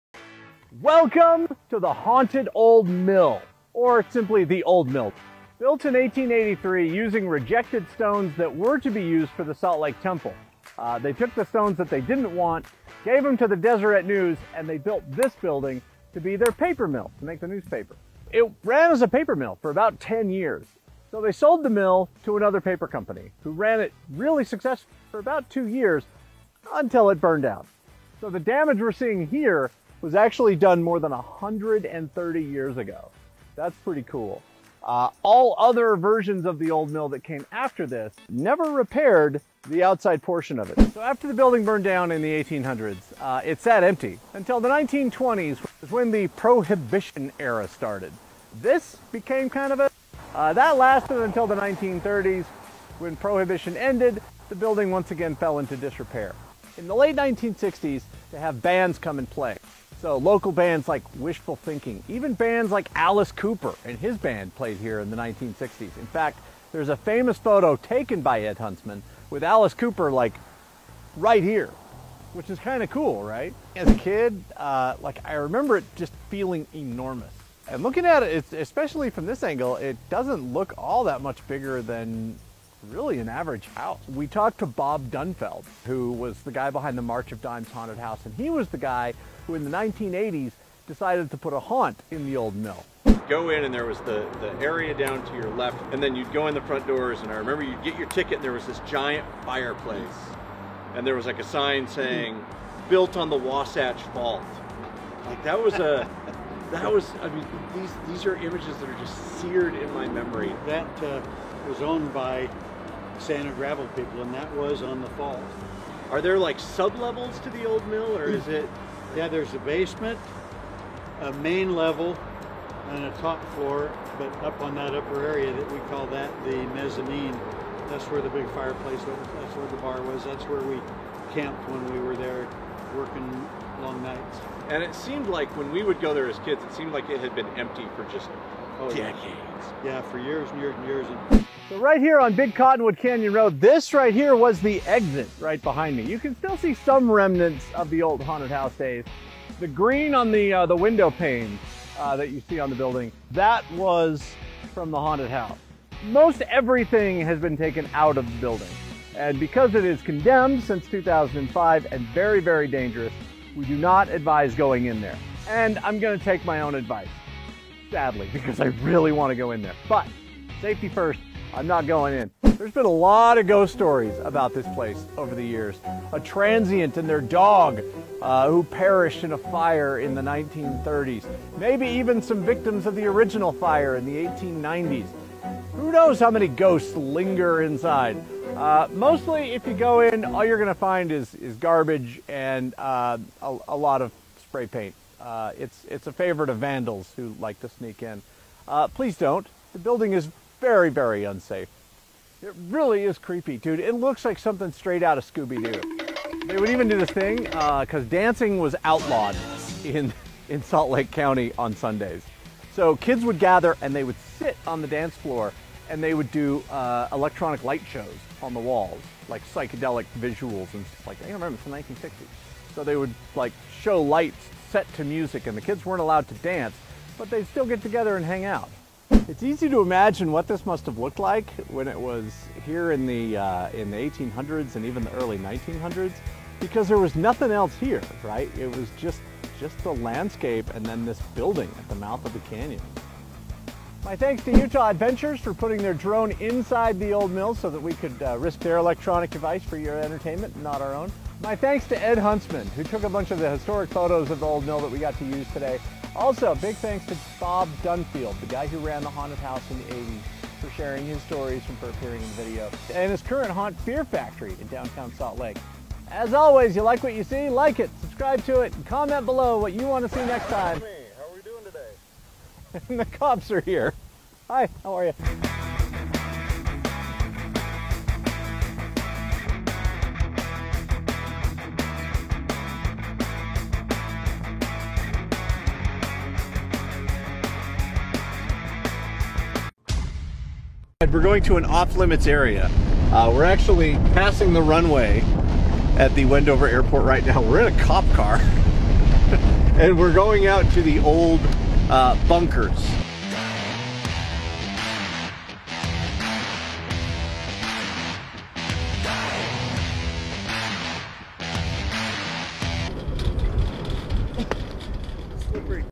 COOL STUFF IN UTAH (Ep 45) presents - The Old Mill
Have you ever wondered what happened to the Old Mill?

Rumor has it that it's haunted!!

Check out this episode of Cool Stuff in Utah where Jon Smith takes you INSIDE and tells you about the Old Mill's history!

Where would you like to see us go for the net episode of Cool Stuff in Utah?